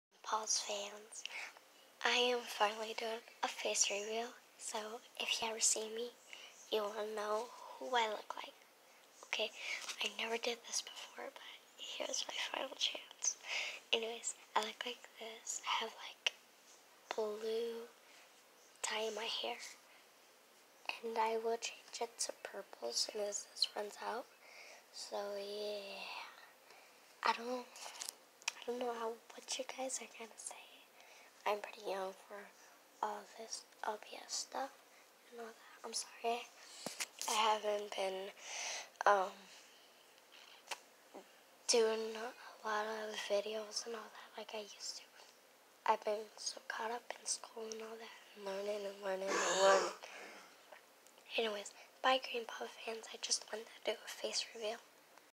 FACE REVIL
First time in ever if u guys see me anywhere I look like this HUG ME GUYS IF U SEE ME EEK